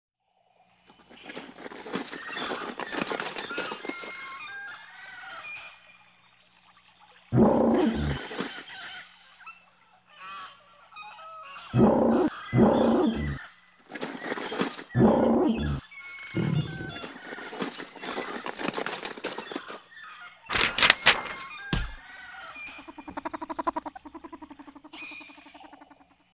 Stop Motion Animation Origami
Origami dinosaur tries to reach an apple from a tree.